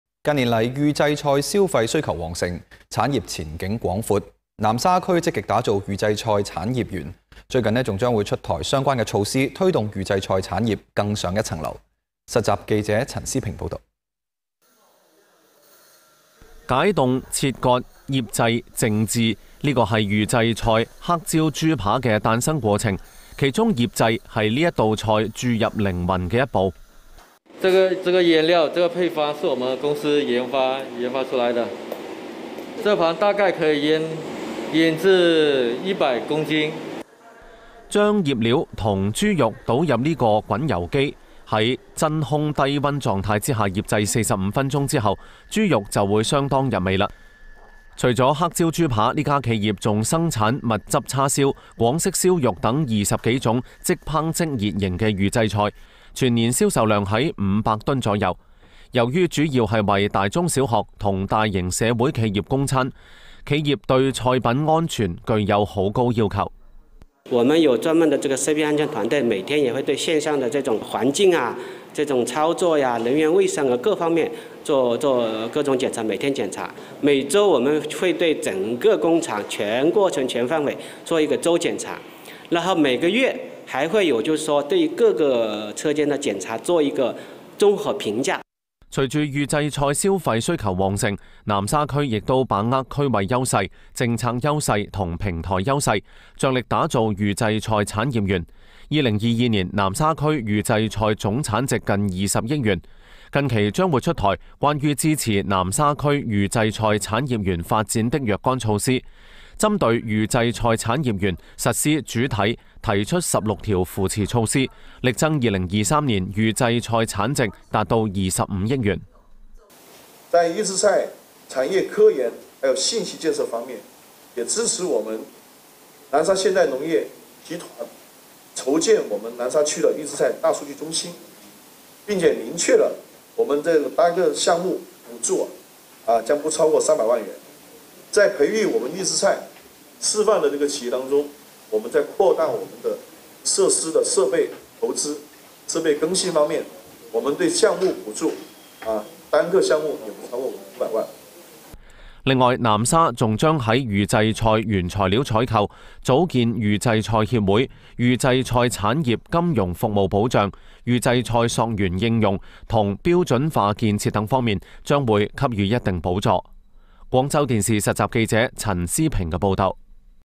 南沙：把預製菜做成「硬菜」
近年來，預製菜消費需求旺盛，產業前景廣闊。南沙區積極打造預製菜產業園，最近還將出台相關措施，推動預製菜產業「更上一層樓」。